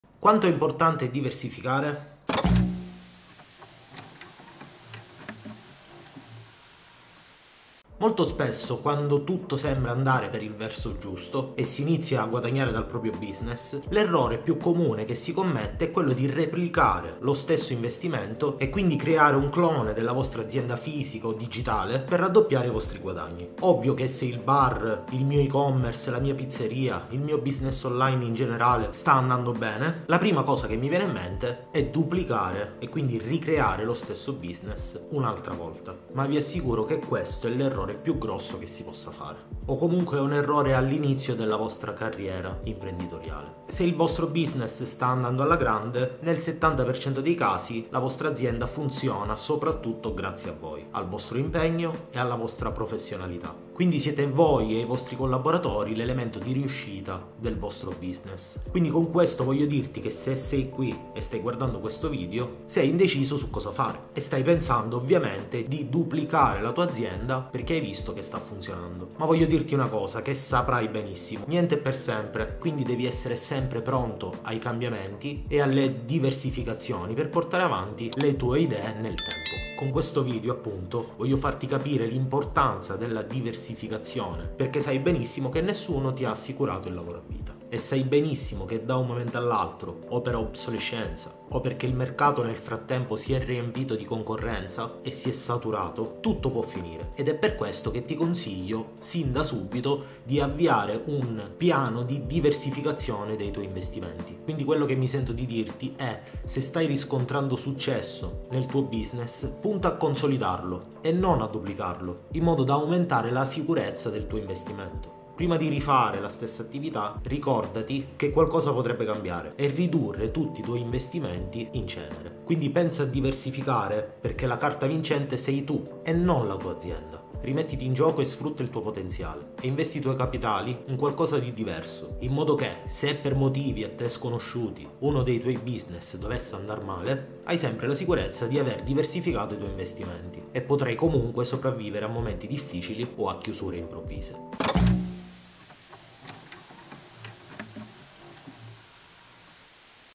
Diversificare i tuoi investimenti assicura i tuoi capitali
La diversificazione è fondamentale quando fai impresa o investimenti in generale. L’errore più comune è puntare tutto sul cavallo vincente ma sopratutto all’inizio della tua carriera questo può farti perdere tutto quindi ti spiego quanto è importate diversificare.
- Questo è il sito che mi permette di creare le mie mappe mentali
- Questa è la fotocamera che uso per girare i miei video in 4k